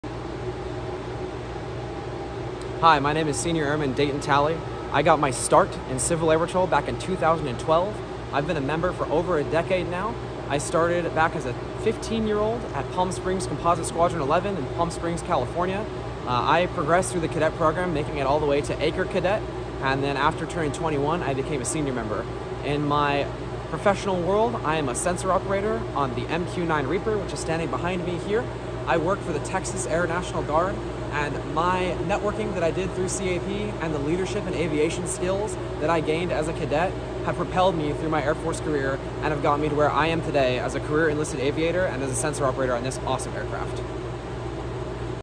Civil Air Patrol Cadet Alum Shares His Story
Civil Air Patrol cadet alum and Air Force Senior Airman Deyton Talley, an MQ-9 Reaper sensor operator from the Texas Air National Guard 147th Attack Wing, recently demonstrated the military unmanned aerial vehicle to teachers attending the Space Exploration Educators Conference in Houston. Talley got his start in aviation in Civil Air Patrol as a cadet with our California Wing’s 11th Composite Squadron. He transitioned from cadet to a CAP officer in 2018. Now, Talley serves as a CAP technical sergeant in our Texas Wing.